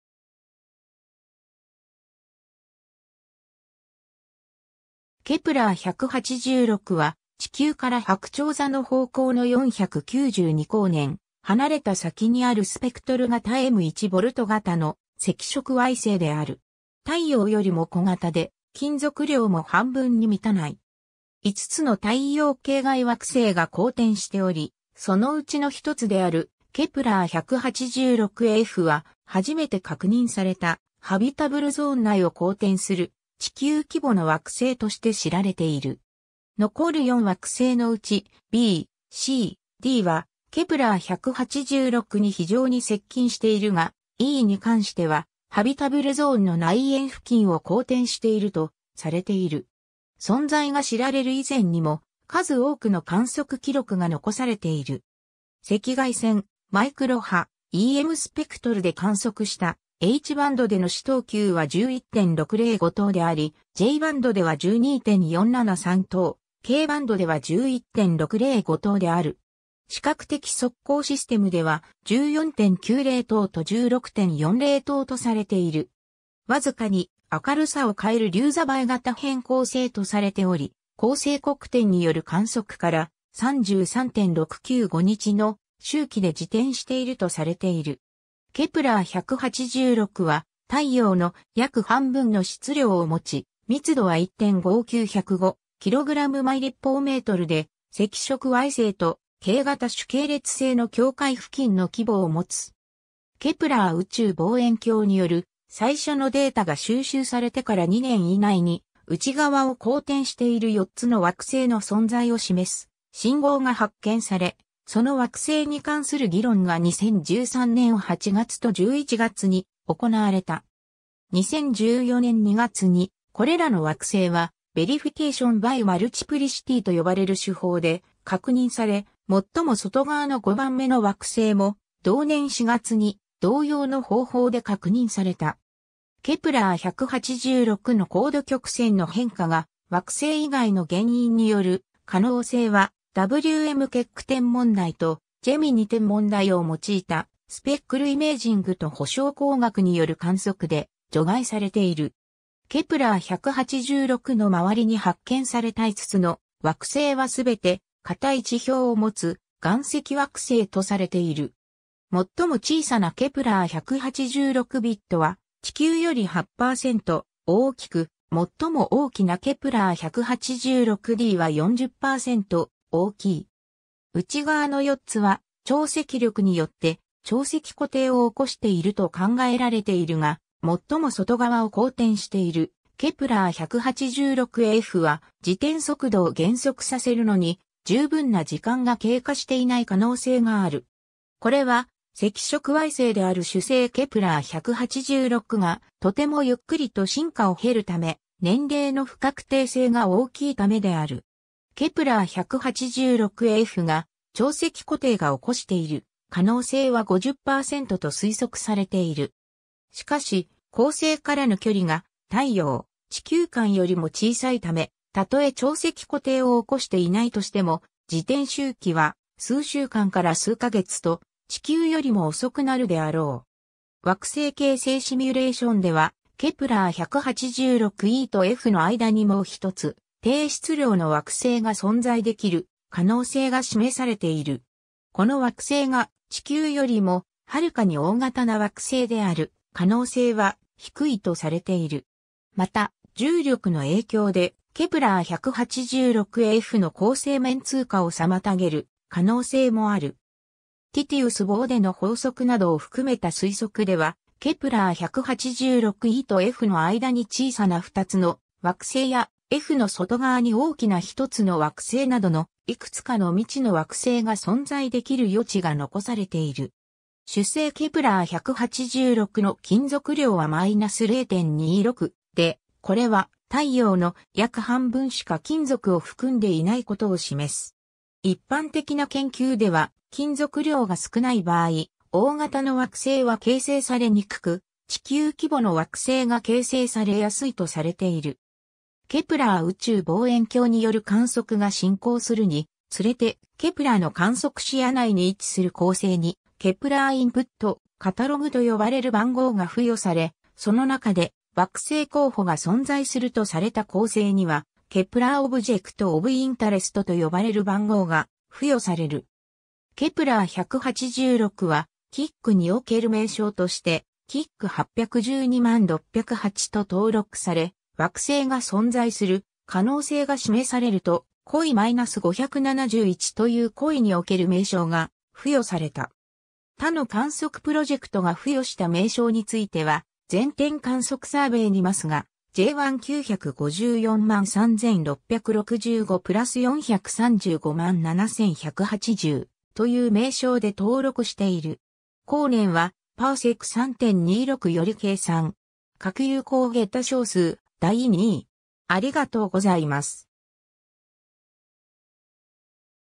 ケプラー186
ケプラー186（英語: Kepler-186）は、地球からはくちょう座の方向の492光年離れた先にあるスペクトル型M1V型の赤色矮星である。
太陽よりも小型で、金属量も半分に満たない。
5つの太陽系外惑星が公転しており、そのうちの一つであるケプラー186fは、初めて確認されたハビタブルゾーン内を公転する地球規模の惑星として知られている。
残る4惑星のうち、b、c、dはケプラー186に非常に接近しているが、eに関してはハビタブルゾーンの内縁付近を公転しているとされている。
 存在が知られる以前にも、数多くの観測記録が残されている。
赤外線/マイクロ波EMスペクトルで観測したHバンドでの視等級は11.605等であり、Jバンドでは12.473等、Kバンドでは11.605等である。
視覚的測光システムでは、14.90等（可視光スペクトルの赤くなっている側）と16.40等（可視光スペクトルの青くなっている側）とされている。
わずかに明るさを変えるりゅう座BY型変光星とされており、恒星黒点による観測から、33.695日の周期で自転しているとされている。
 ケプラー186は太陽の約半分の質量を持ち、密度は~1.59×105 kg/m3で、赤色矮星とK型主系列星の境界付近の規模を持つ。
 ケプラー宇宙望遠鏡による最初のデータが収集されてから2年以内に、内側を公転している4つの惑星の存在を示す信号が発見され、その惑星に関する議論が2013年8月と11月に行われた。
2014年2月に、これらの惑星はVerification by multiplicity（多様性による検証）と呼ばれる手法で確認され、最も外側の5番目の惑星も、同年4月に同様の方法で確認された。
ケプラー186の光度曲線の変化が、惑星以外の原因による可能性はW・M・ケック天文台とジェミニ天文台を用いたスペックル・イメージングと補償光学による観測で除外されている。
 ケプラー186の周りに発見された5つの惑星は全て、堅い地表を持つ岩石惑星とされている。
最も小さなケプラー186bは地球より8%大きく、最も大きなケプラー186dは40%大きい。
 内側の4つは、潮汐力によって潮汐固定を起こしていると考えられているが、最も外側を公転しているケプラー186fは、自転速度を減速させるのに十分な時間が経過していない可能性がある。
これは、赤色矮星である主星ケプラー186がとてもゆっくりと進化を経るため、年齢の不確定性が大きいためである。
ケプラー186fが潮汐固定が起こしている可能性は50%と推測されている。
しかし、恒星からの距離が太陽～地球間よりも小さいため、たとえ潮汐固定を起こしていないとしても、自転周期は数週間から数ヶ月と、地球よりも遅くなるであろう。
 惑星形成シミュレーションでは、ケプラー186eとfの間にもう1つ、低質量の惑星が存在出来る可能性が示されている。
この惑星が、地球よりもはるかに大型な惑星である可能性は低いとされている。
また、重力の影響で、ケプラー186fの恒星面通過（トランジット）を妨げる可能性もある。
ティティウス・ボーデの法則などを含めた推測では、ケプラー186eとfの間に小さな2つの惑星や、fの外側に大きな1つの惑星などの、いくつかの未知の惑星が存在出来る余地が残されている。
 主星ケプラー186の金属量は-0.26で、これは太陽の約半分しか金属を含んでいない事を示す。
一般的な研究では、金属量が少ない場合、大型の惑星は形成されにくく、地球規模の惑星が形成されやすいとされている。
 ケプラー宇宙望遠鏡による観測が進行するにつれて、ケプラーの観測視野内に位置する恒星にKepler Input Catalog（KIC）と呼ばれる番号が付与され、その中で、惑星候補が存在するとされた恒星にはKepler object of interest（KOI）と呼ばれる番号が付与される。
ケプラー186は、KICにおける名称として、KIC 8120608と登録され、惑星が存在する可能性が示されるとKOI-571というKOIにおける名称が付与された。
 他の観測プロジェクトが付与した名称については、全天観測サーベイ2MASSがJ19543665+4357180という名称で登録している。
 ^ 光年はパーセク×3.26より計算。
各有効桁小数第2位